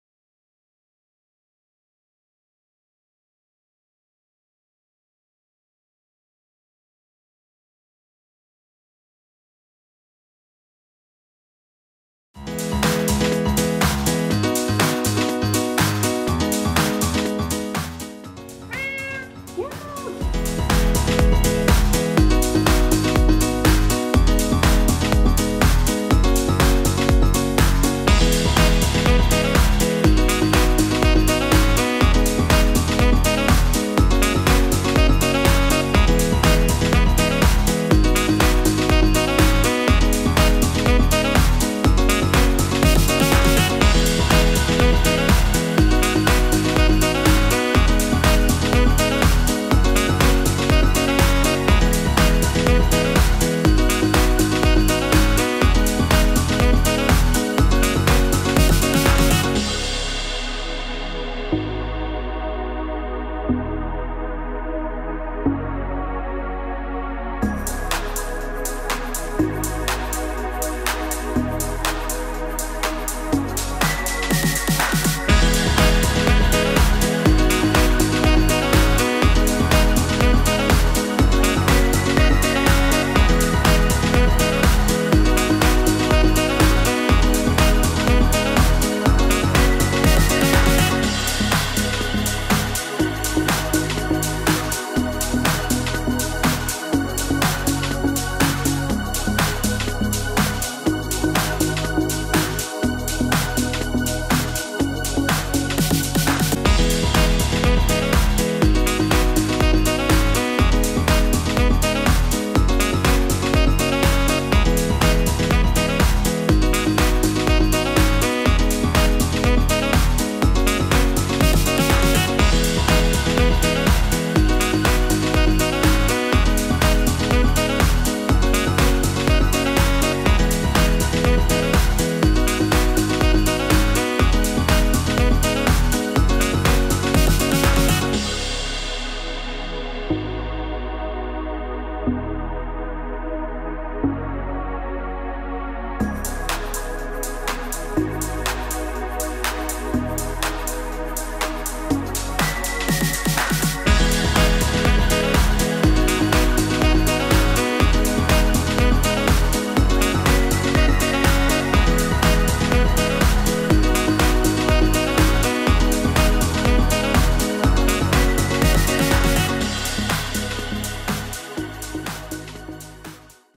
How to get a Bengal Cat to wear a Tie | First Time
Our Bengal Cat wears a tie for the first time. How to let your cat play dress up? Playing dress up with cats is not as easy as dogs. These ties make it super easy to quickly put it on your cats neck and are super cute.. Here’s how:
Step 1: Find cat
Step 2: Undo velcro straps
Step 3: Quickly but gently fastened the velcro straps around cat’s neck.

Amazon link-
Cat ties and bow-tie